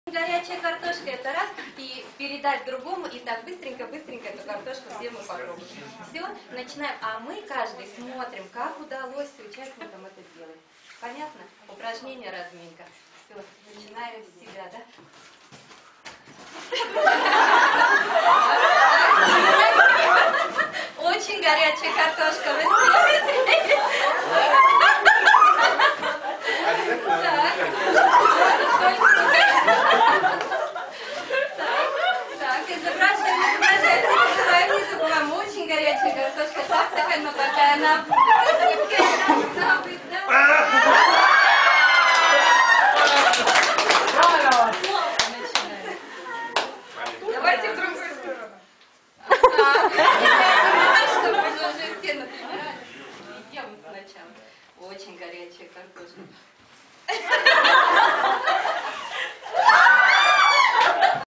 Игра Горячая Картошка
Игра на знакомство под названием Горячая Картошка. очень весёлая и смешная.